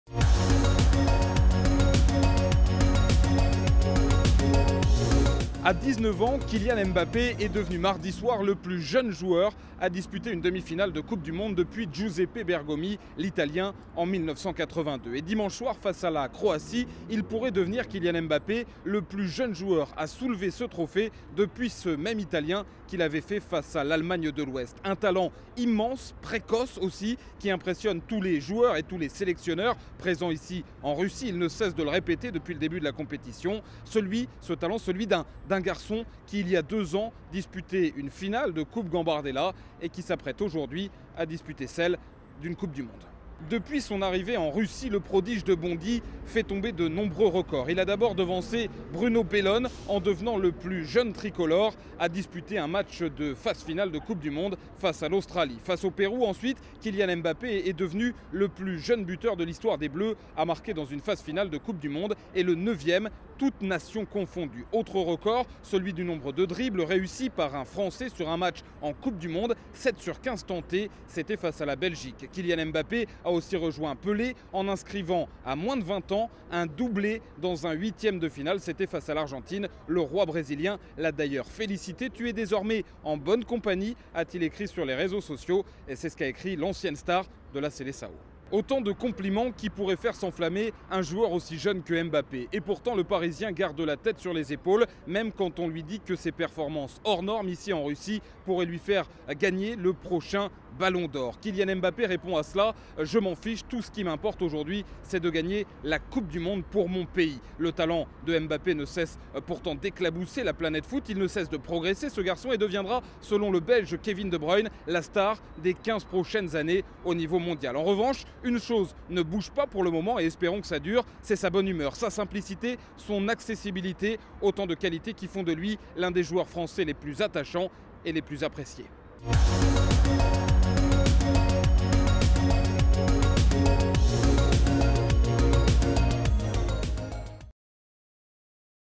Un jour, un joueur : Kylian Mbappé
Le jeune (19 ans) joueur de l'équipe de France est l'une des vedettes de la Coupe du monde 2018. Il sera, selon le Belge Kevin De Bruyne, la star des 15 prochaines années.